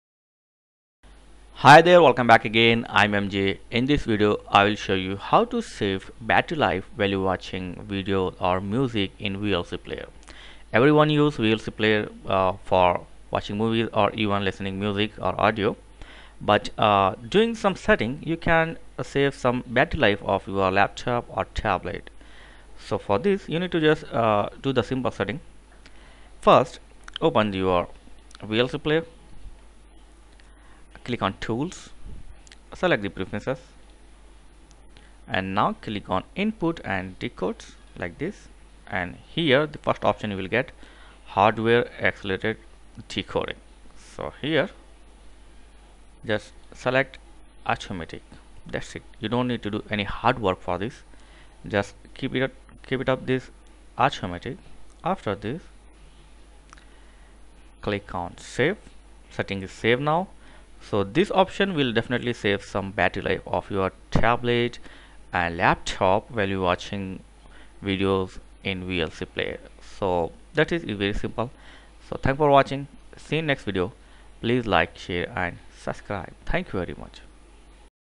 How to Save Battery While Watching in VLC Player
Save some battery life of your laptop or tablet while watching movies in vlc player